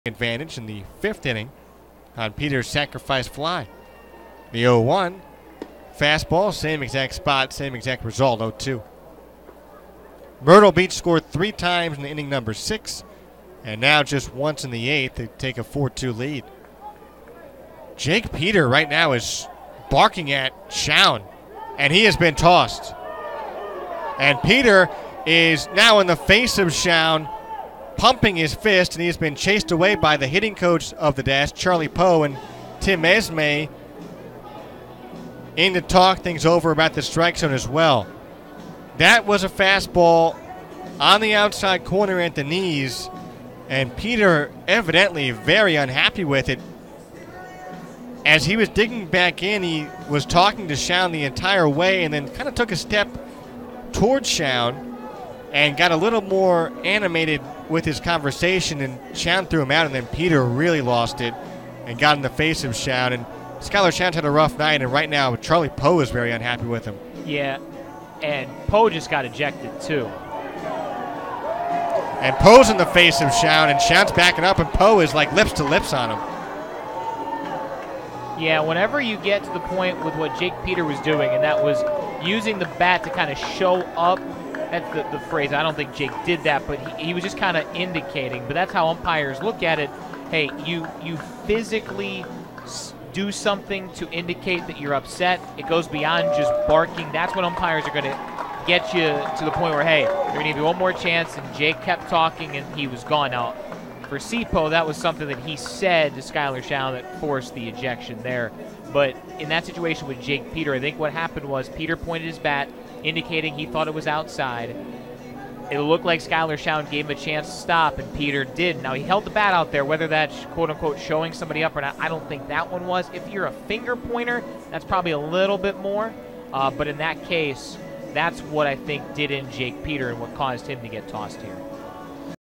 Dash's Peter, Poe get ejected
6/17/15: Winston-Salem's Jake Peter and manager Charles Poe get ejected after arguing with umpire Skyler Shown